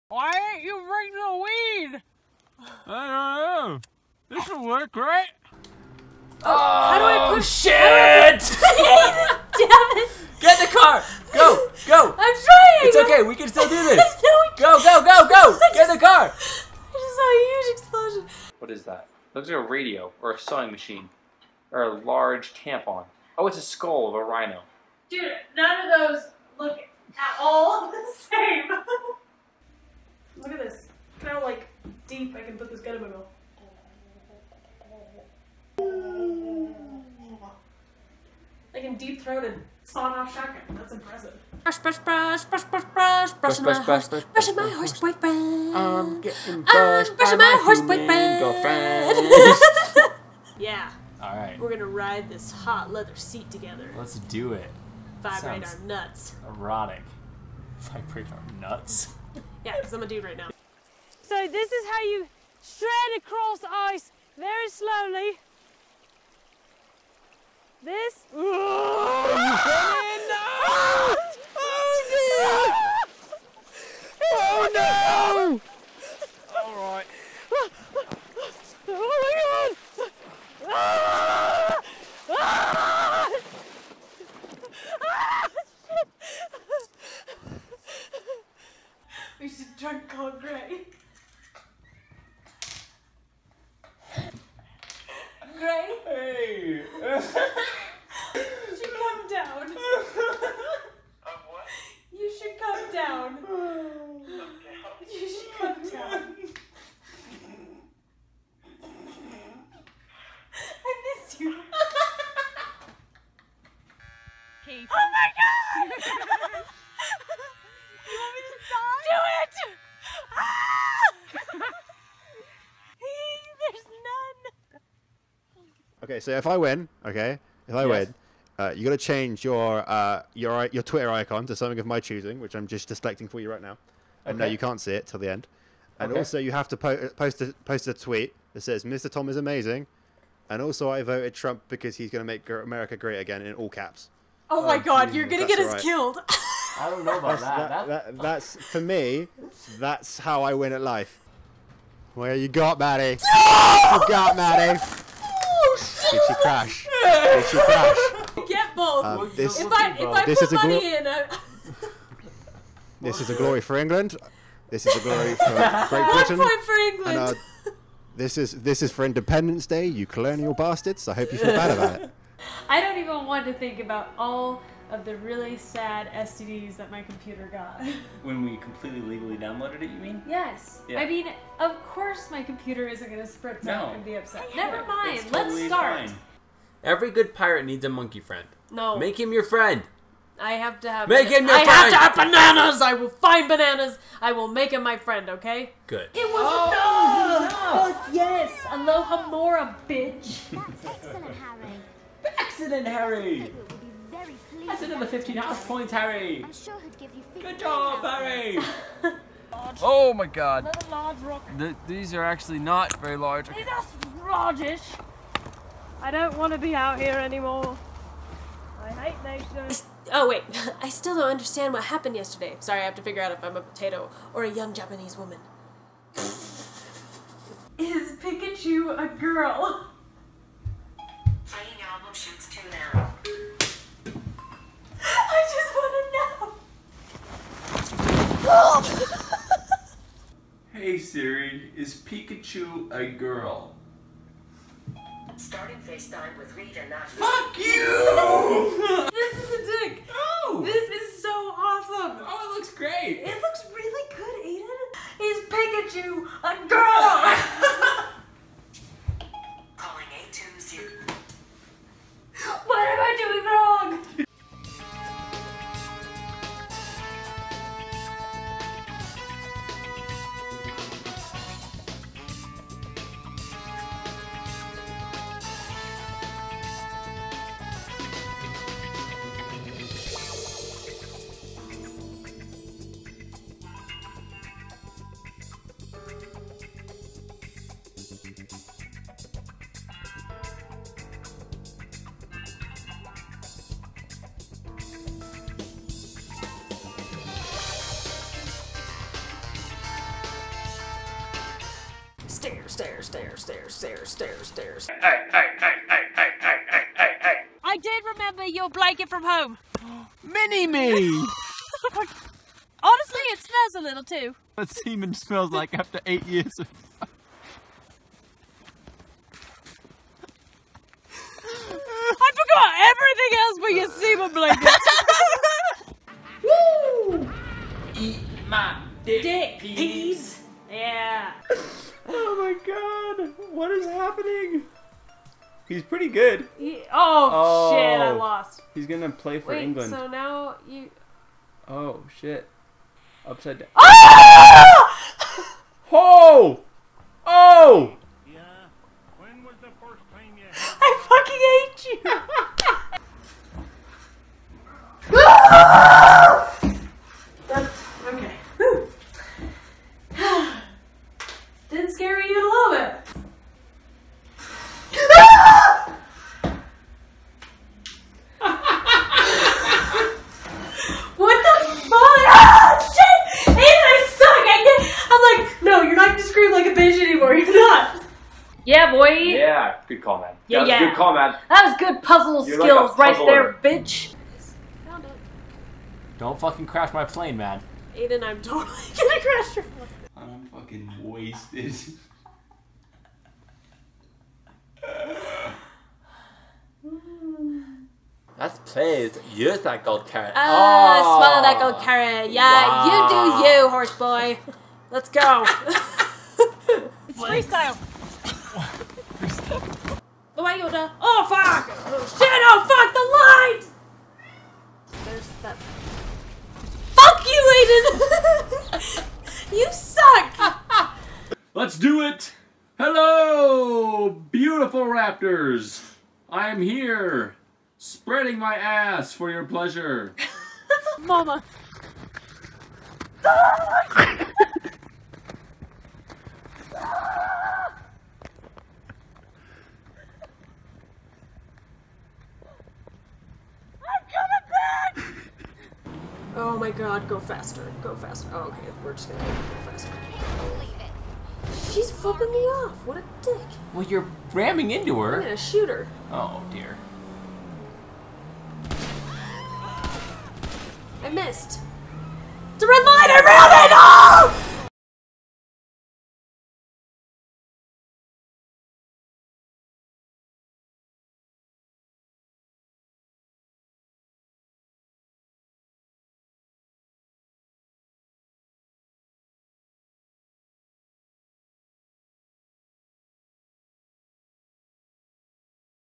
300th Video Highlight Spectacular!!
In this video we celebrate 300 crazy, outrageous videos, by looking back at some of our favorite moments.
Comboninjas: @comboninjas
Aidan: @aidanjamesr
Madi: @madisonngarza